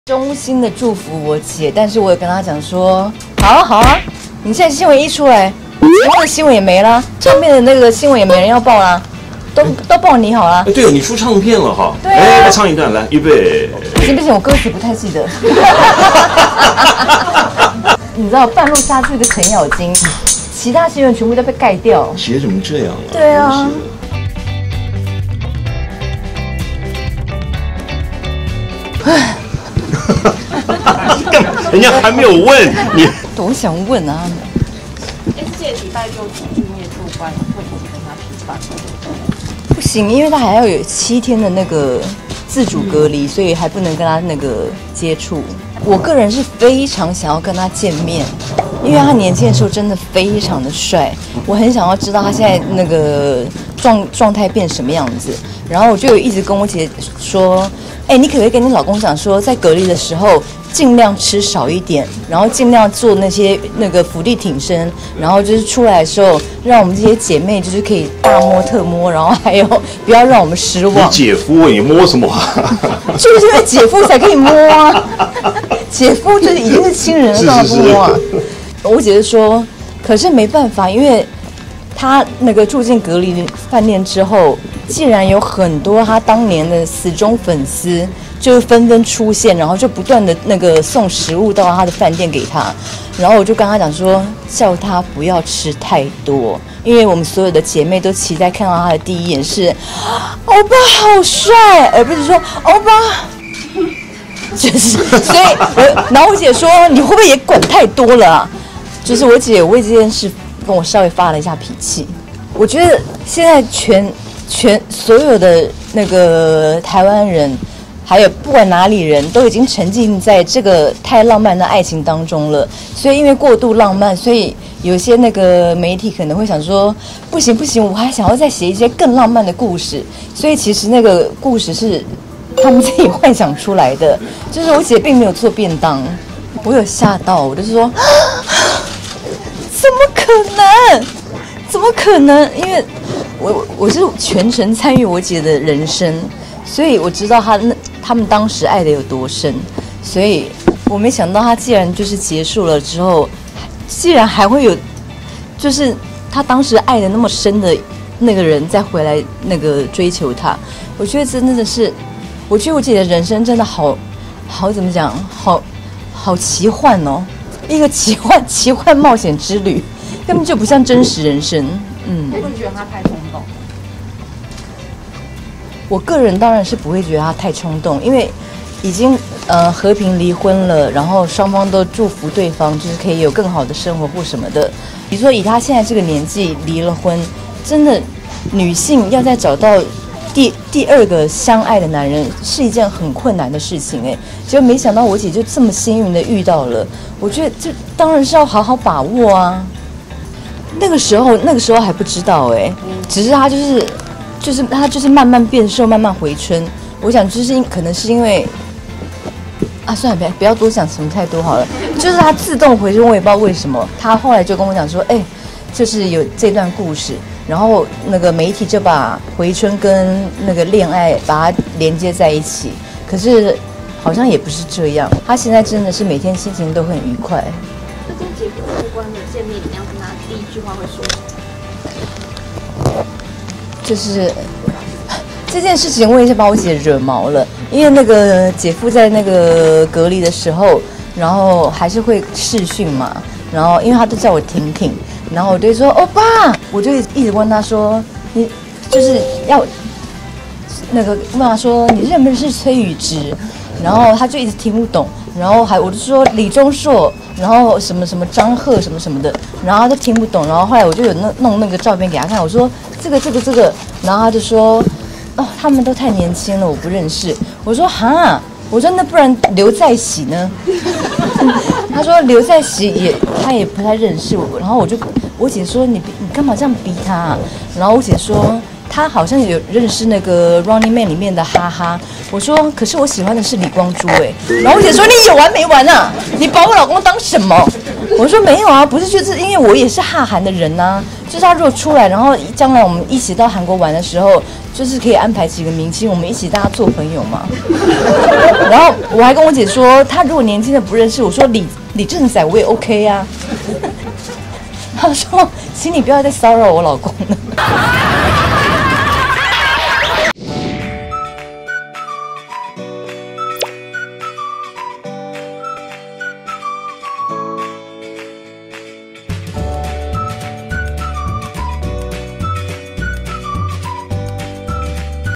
【撩星聞】小姨子發夢 摸透新姊夫具俊曄惹毛大S 「不要騷擾我老公！」
小S徐熙娣全程見證大S與具俊曄的舊情復燃，表示認為大S可以找到第二春時十分難得，更希望可以透過姊夫和韓國藝人當朋友，也笑說姊夫出關後，希望可以「大摸特摸」！